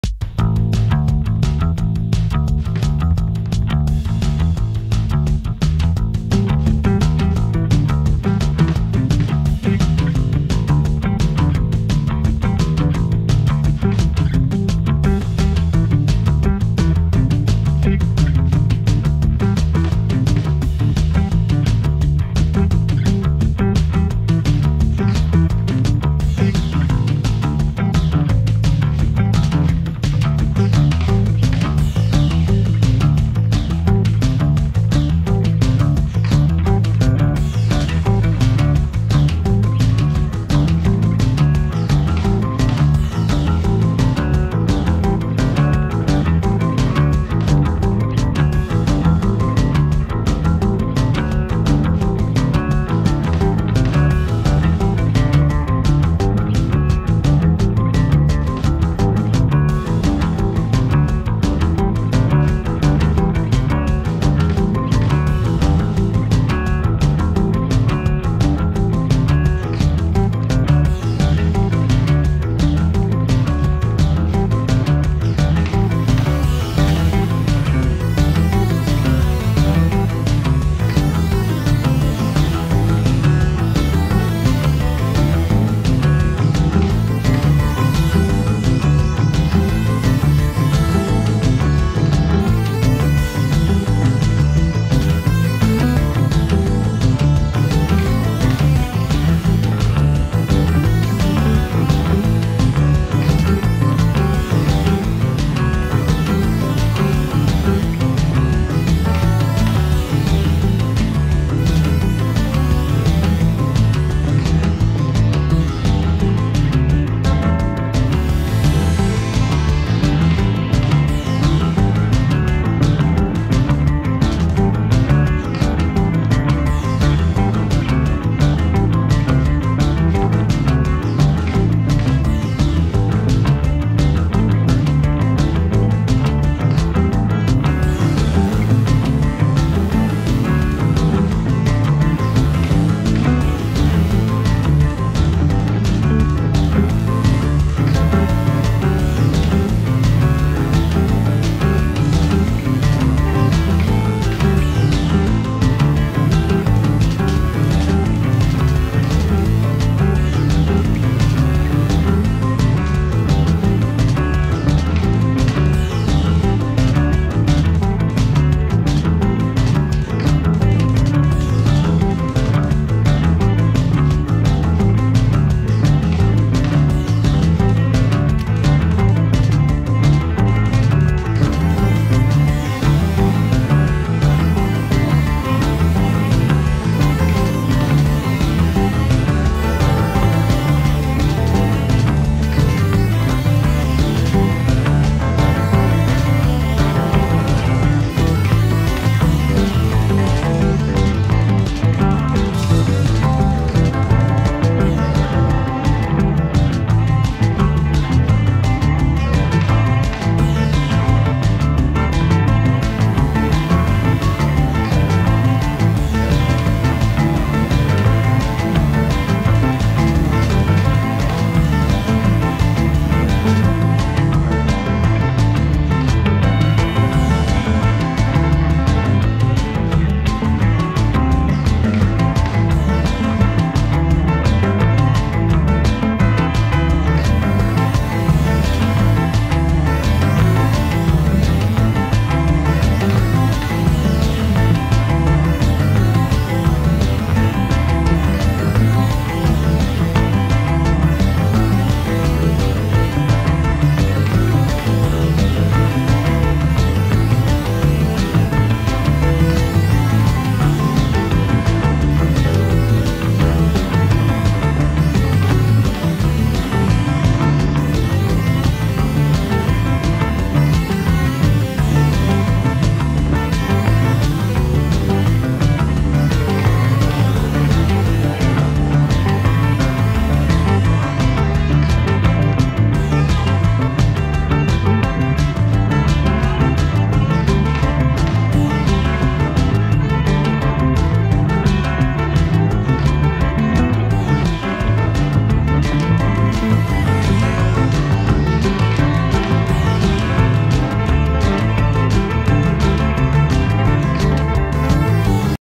FPV Drone Crash Compilation - Fails
A Compilation of my Fpv drone crashes and fails. I crashed many drones and had ro repair so many times.